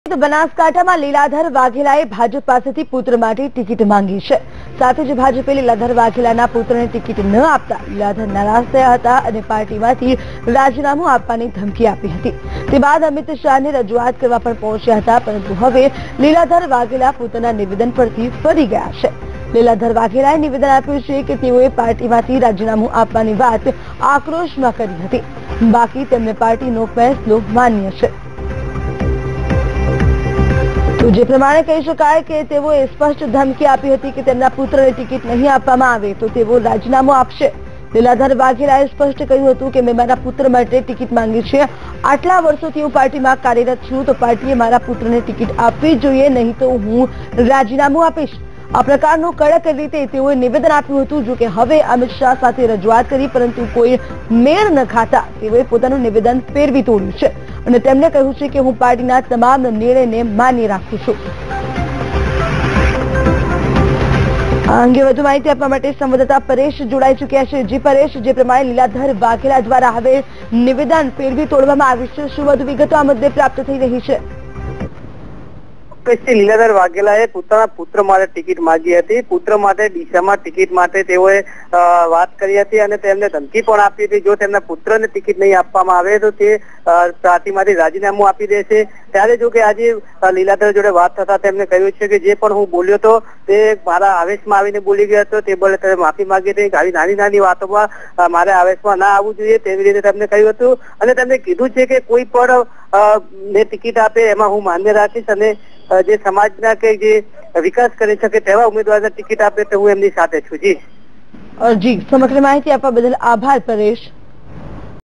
Gujarat Elections: BJP MP Liladhar Vaghela takes back his warning on son's ticket | Vtv News
Gujarat Elections: BJP MP Liladhar Vaghela takes back his warning on son's ticket .